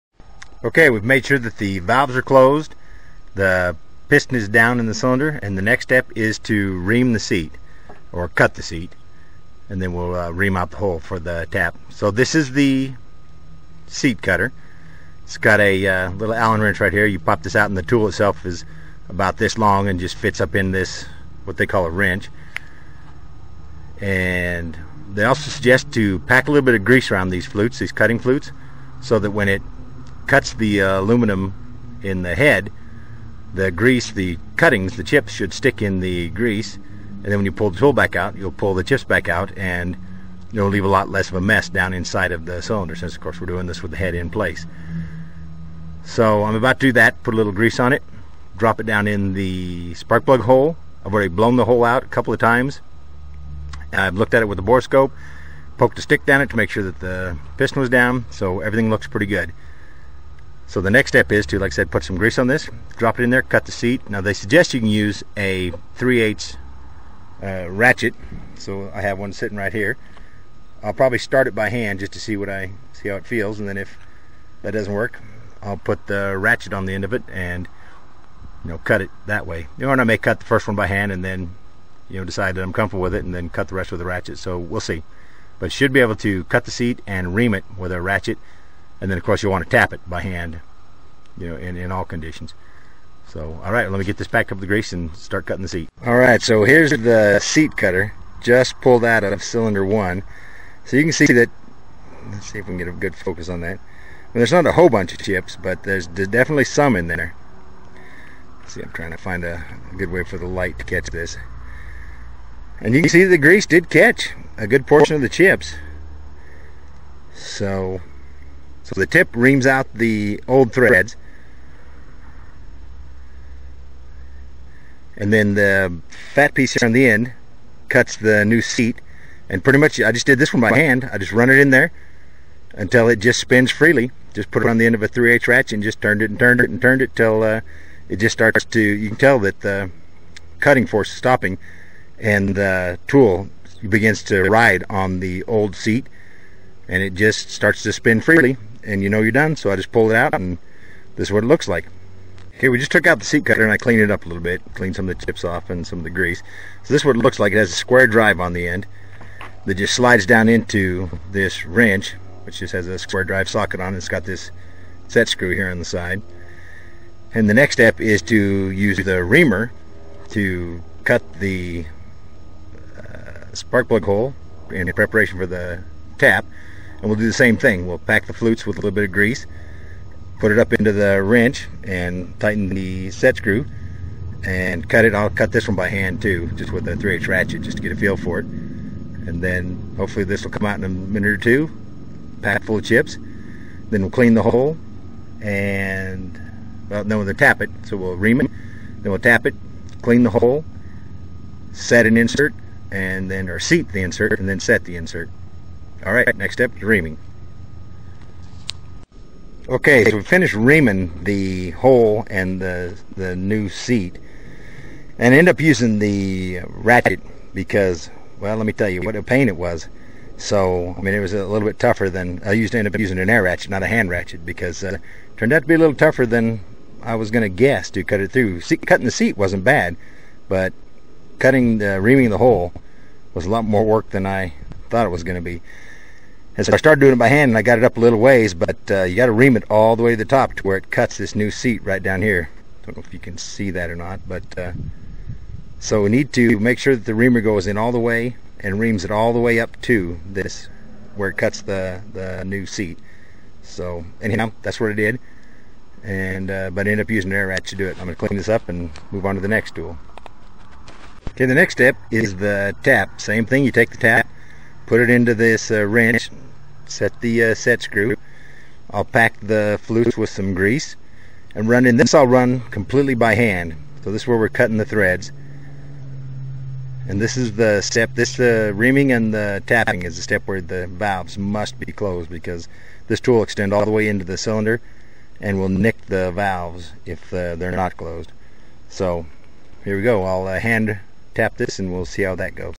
33 37 Cutting the new seat, reaming and tapping for the spark plug insert
Cutting, Reaming, and Tapping the spark plug holes for the inserts